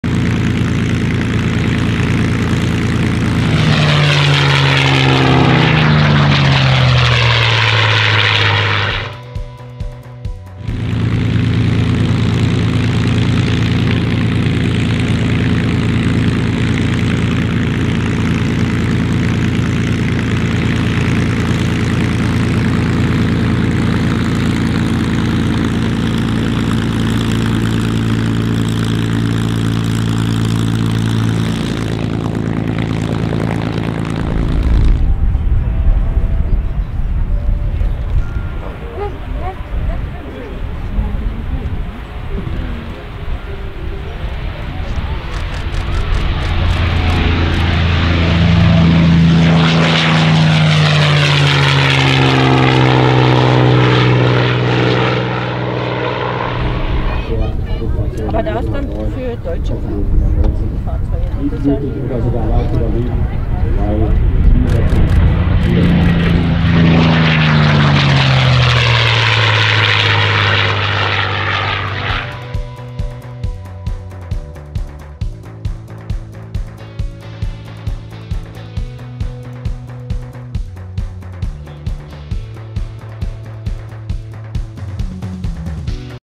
North American P-51D Mustang at Stearman & Friends 2022 #01
Stearman & Friends 2022
Bienenfarm (EDOI), Germany

NL51ZW, North American P-51D Mustang, Flying Legends

A board game about an airport? Check it out :)